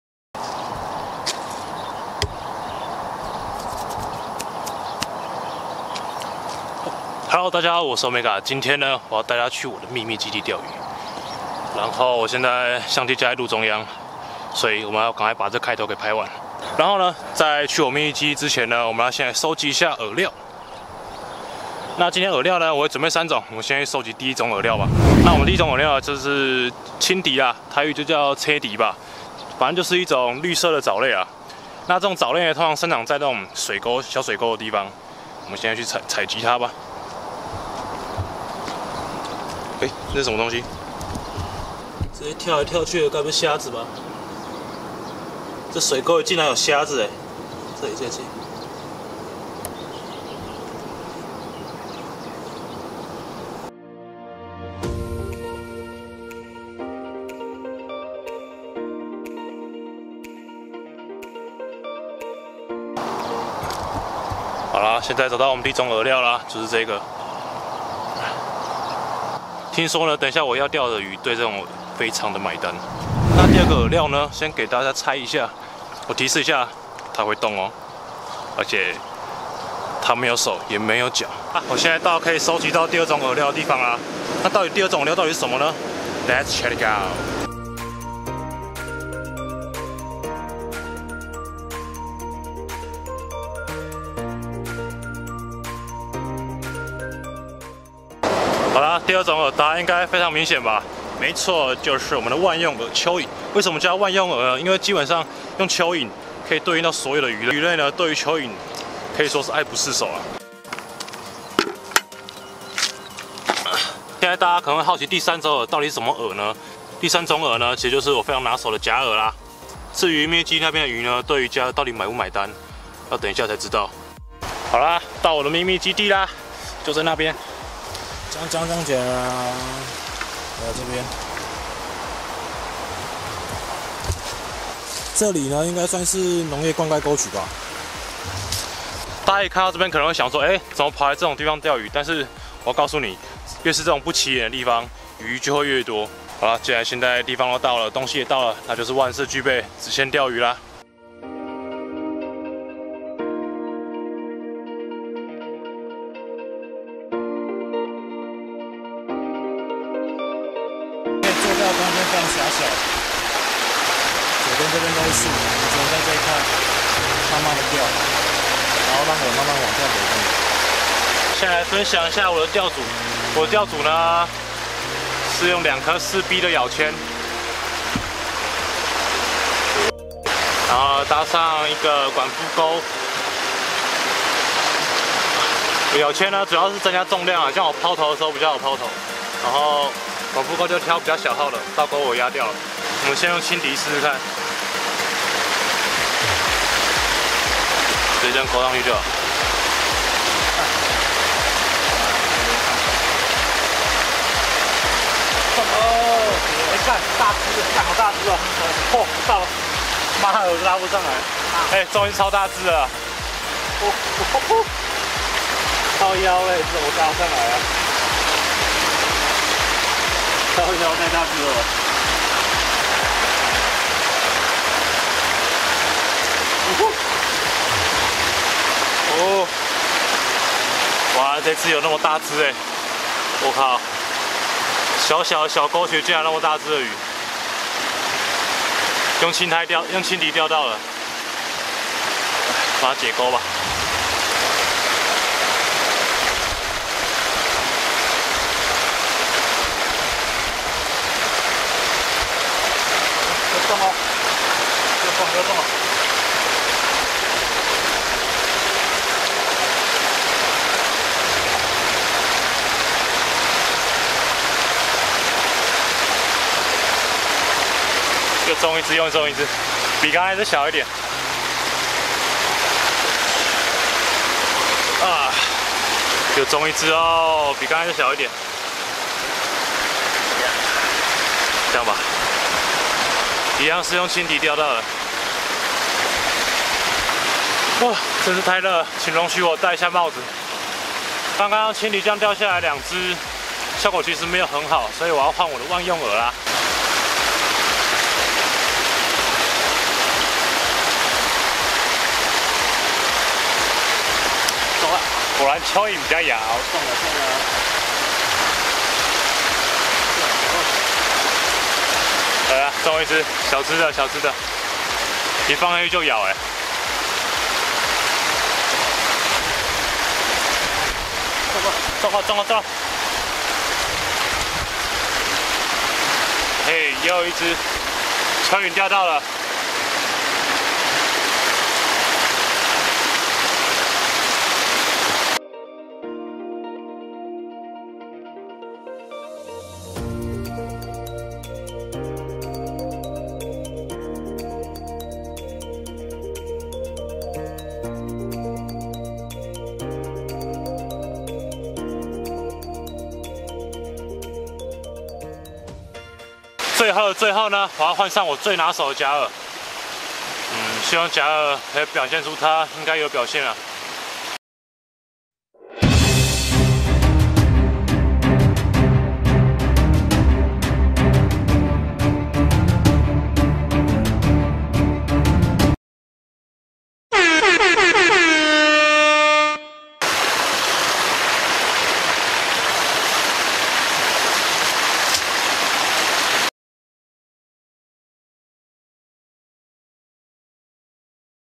秘密基地！！在一個極度隱密的灌溉溝渠裡釣魚！！(前打/路亞/磯釣/吳郭魚)
好久沒發片了阿!!
終於有時間可以拍片了 各位久等了
獻上這部輕鬆小品